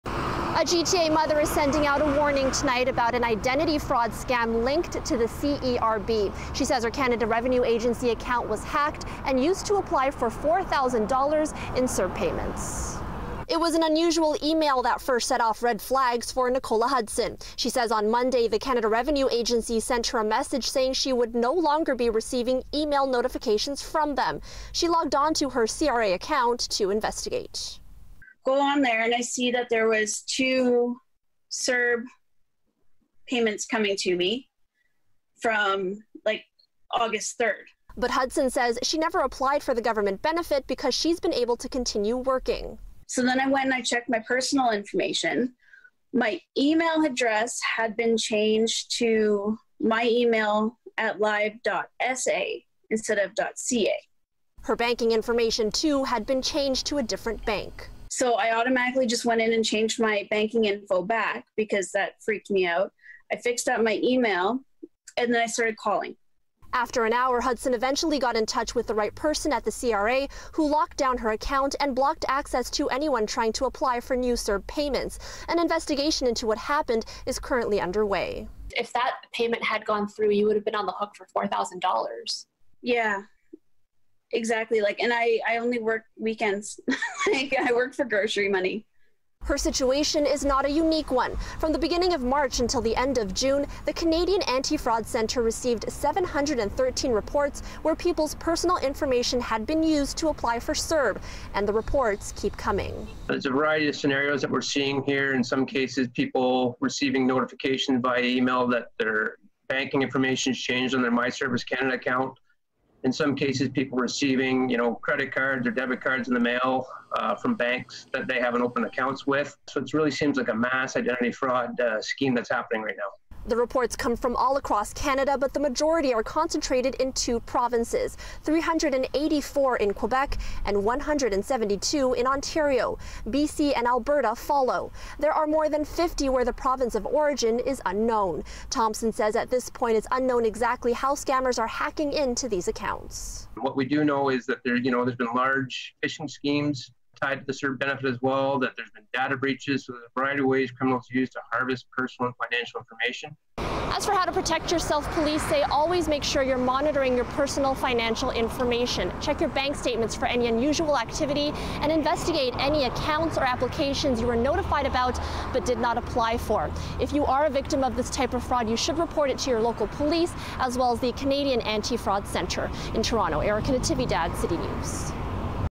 GTA mother warns of identity fraud linked to CERB
A GTA mother is issuing a public warning after her Canada Revenue Agency account was used to apply for $4000 in CERB. As Erica Natividad reports, the woman logged in to find her bank account number and email changed.